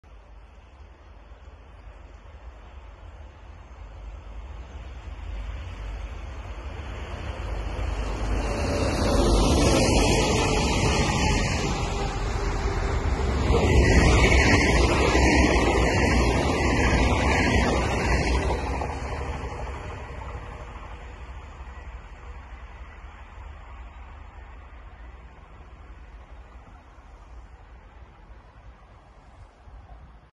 15 Augustus 2021 Helmond: Railadventure 9903 Met de 188 005 en de 188 006
15 Augustus 2021 Helmond: Railadventure/Railexperts 9903 Met de 188 005 en de 188 006. De locomotieven zullen voor Ricardo Rail en RRF toelatingsritten gaan rijden in Nederland